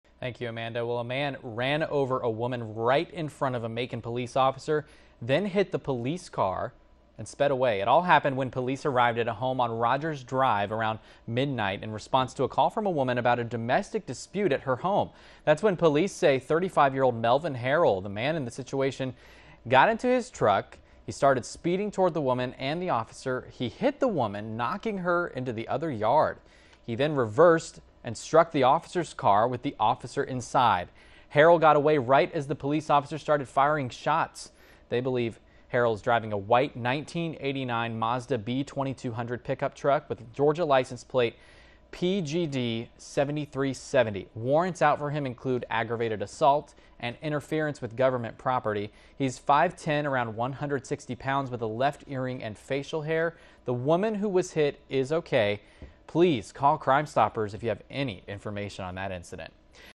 41NBC/WMGT- Suspect Sought- 8.16.13
A man runs over a woman right in front a Macon Police officer then hits the police car and speeds away.